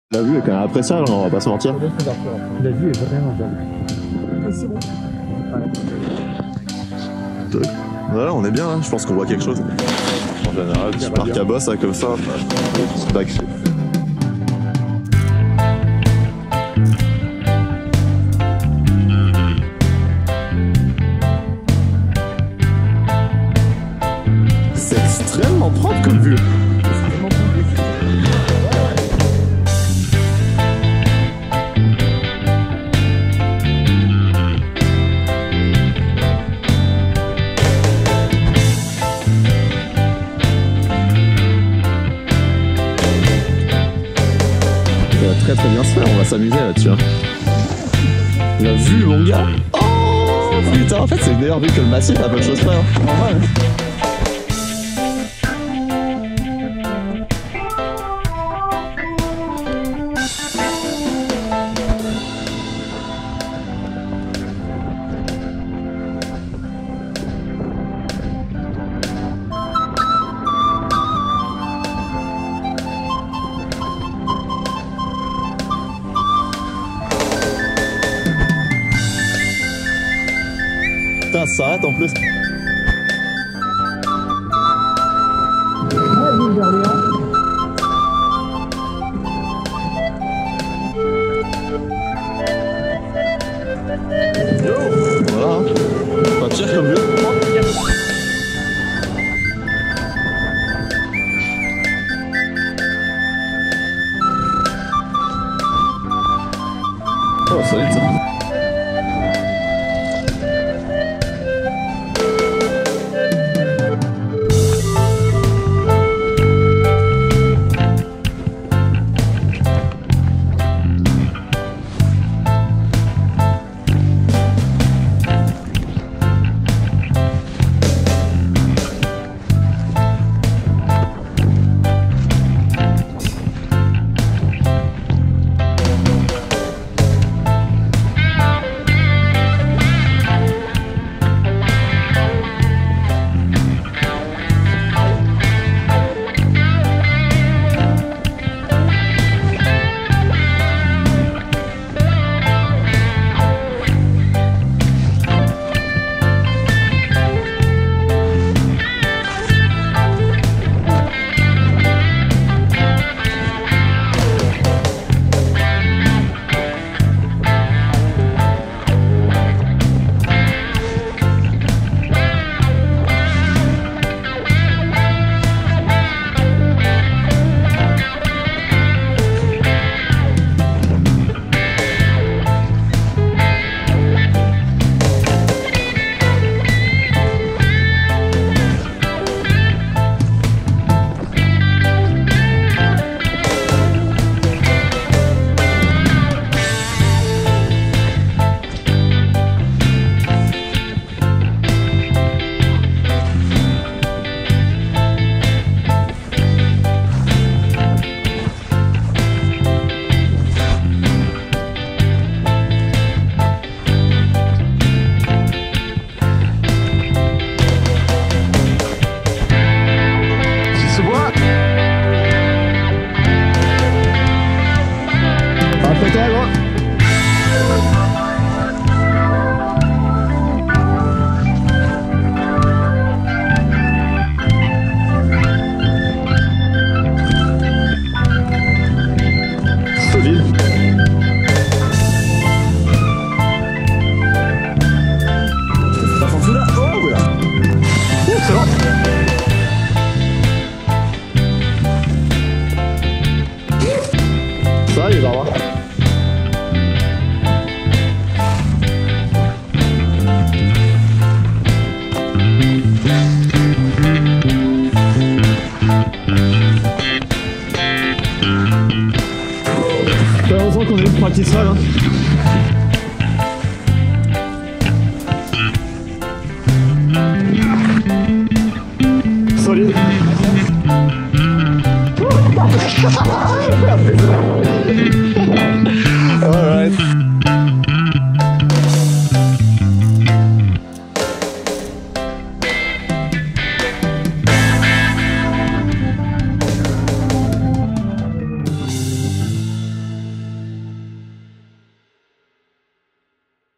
Ski dans la Forêt Enchantée au Mont-Saint-Anne (Original Reggae Music Video)
Session de descente godillée et petit sautillé de bosses sur l'entierté de La Forêt Enchantée pendant une journé magnifique au Mont-Saint-Anne, vues époustouflantes sur le Saint-Laurent et l'Île d'Orléans.

Gros son reggae POSEY composé pour l'occasion en musique de fond.

GoPro HERO 2018